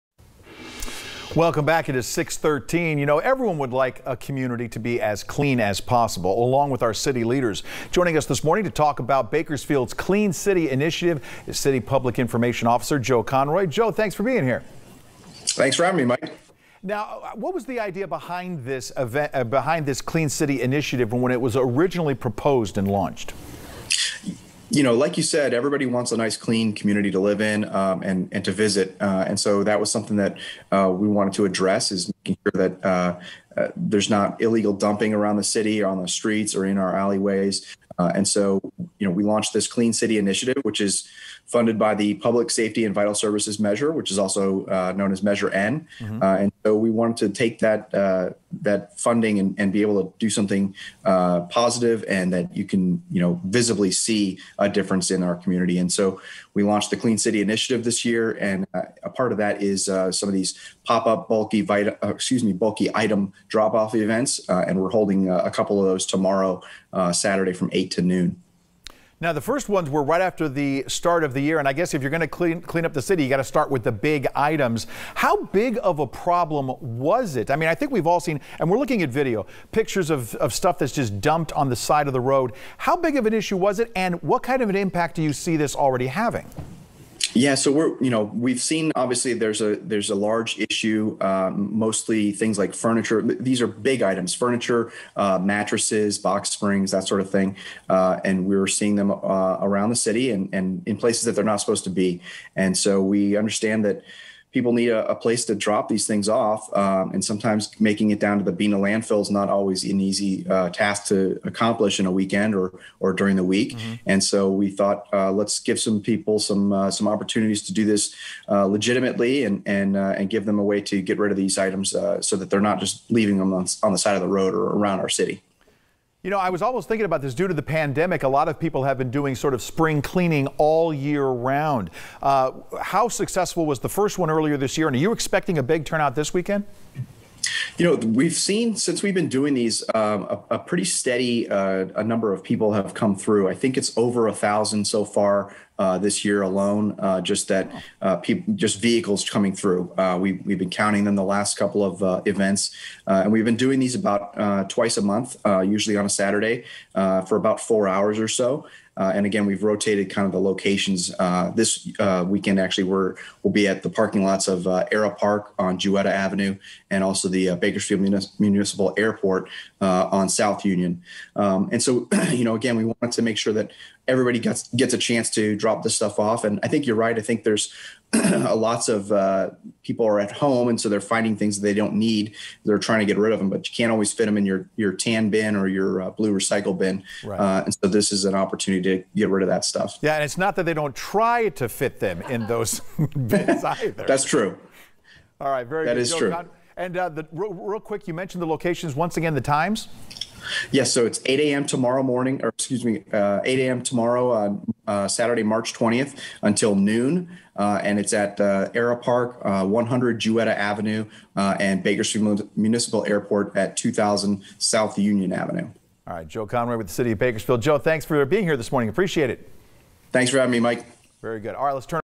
Bakersfield Clean City Initiative: Keeping our environment and city clean
city public information officer Joe Conroy